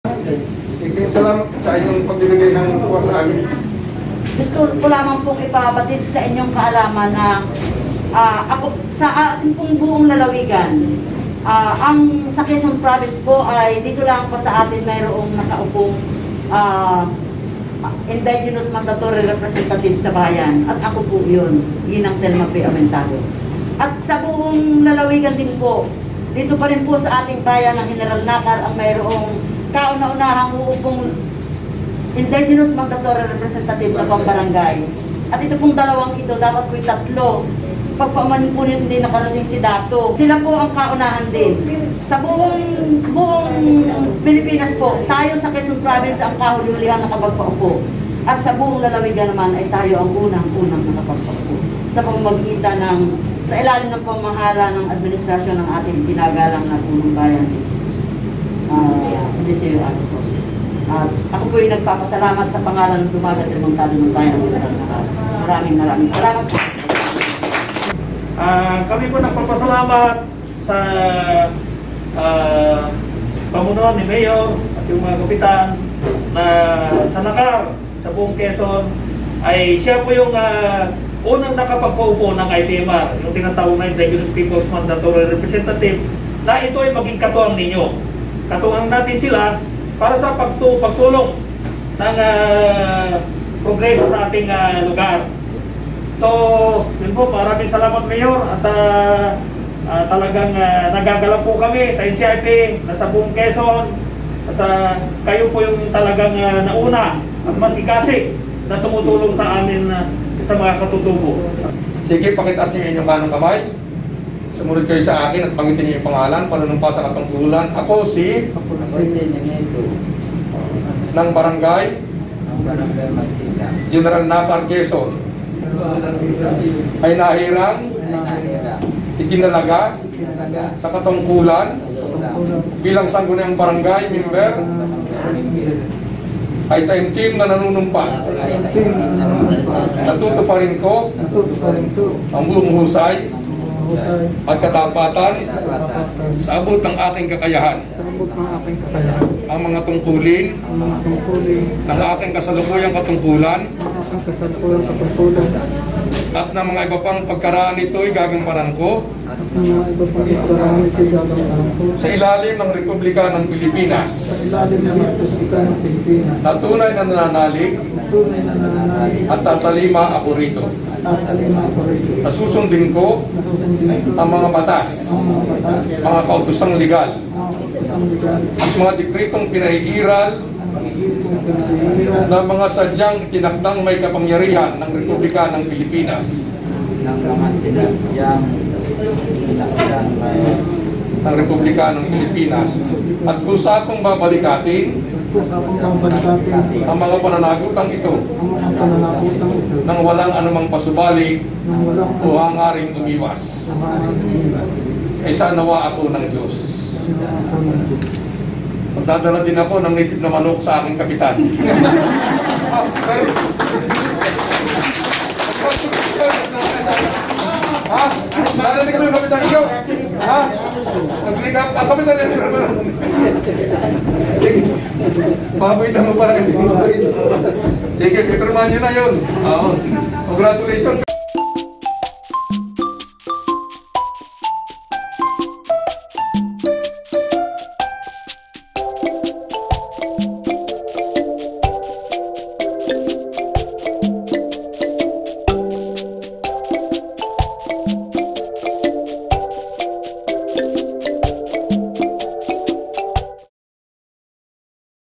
Panunumpa sa Katungkulan ng mga Katutubo | Indigenous People Mandatory Representative
Nanumpa sa katungkulan ngayong araw, ika-6 ng Oktubre, 2021, sa harap ng ating Punong Bayan, Kgg. Esee Ruzol, ang dalawang katutubo na hinirang bilang Indigenous People Mandatory Representative (IPMR) sa mga barangay. Sila ay sina Kgg. Adelo T. Aumentado mula sa Brgy. Sablang at Kgg. Nenito B. Conchada mula sa Brgy. Magsikap.

Kinilala at pinasalamatan naman nina NCIP Quezon Province Provincial Officer Angelo Salidao at Konsehal Thelma Aumentado, IPMR sa Sangguniang Bayan ng General Nakar, ang Ama ng ating bayan, Hon. Mayor Esee Ruzol, dahil sa ilalim ng kanyang pamumuno nagkaroon at nakapaghirang ng IPMR sa ating bayan na syang kauna-unahan sa lalawigan ng Quezon. Mahalaga ang papel ng mga itinalagang IPMR dahil sila ang magiging katuwang ng ating Lokal na Pamahalaan sa pakikipag-ugnayan sa mga kapatid nating katutubo.

Sinaksihan ang nasabing panunumpa sa katungkulan ng mga Punong Barangay at ni MLGOO Alely Ditalo.

Patuloy na pinahahalagahan ng Lokal na Pamahalaan sa pangunguna ng ating Punong Bayan, Kgg. Esee Ruzol, ang tinig ng mga katutubo kaya't ang pagtatalaga ng IPMR sa ating bayan at mga barangay ay susi sa mas maayos na ugnayan tungo sa tunay na kaunlaran ng ating bayan.